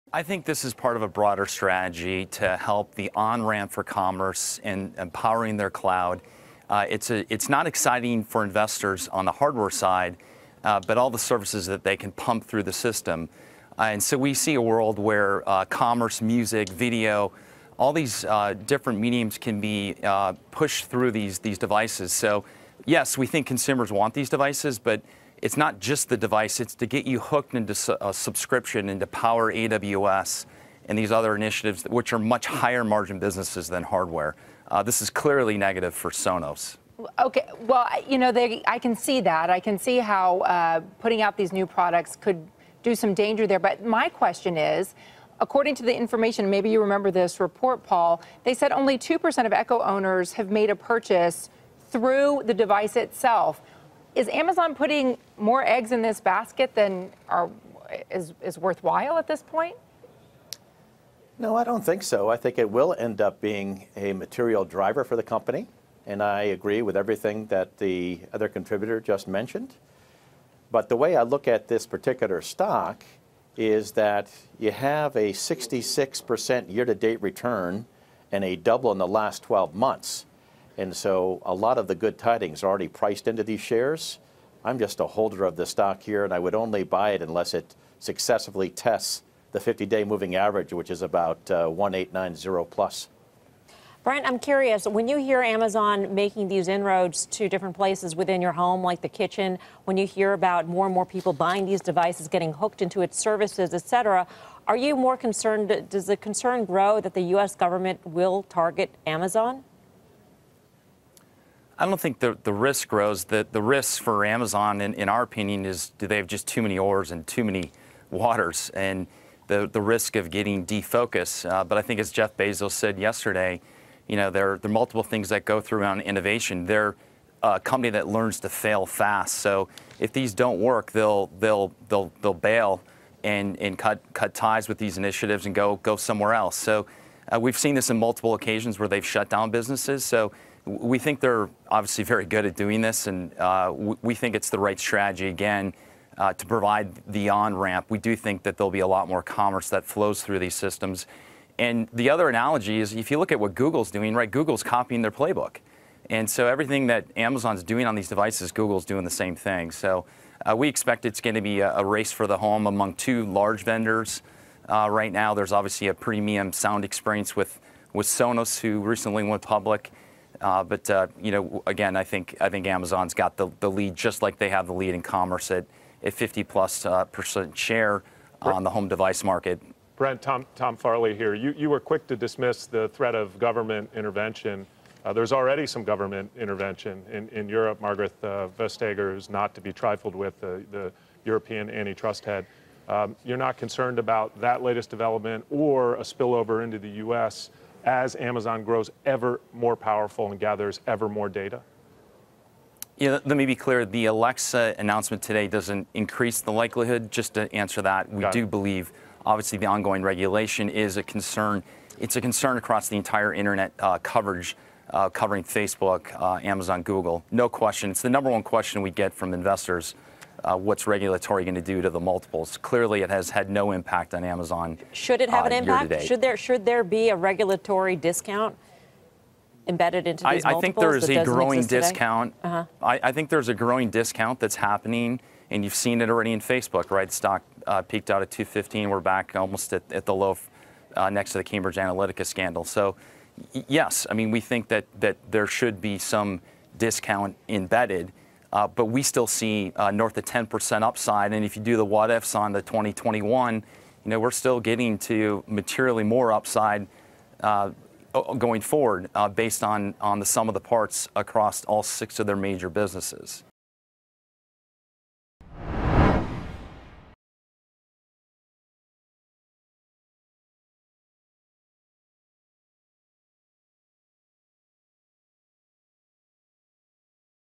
Amazon's devices are for getting consumers into services ecosystem, says analyst
Paul Meeks, Sloy, Dahl & Holst CIO, and Brent Thill, Jefferies senior technology analyst, discuss Amazon's announcement of Alexa-enabled devices and what it means for the tech giant's stock price.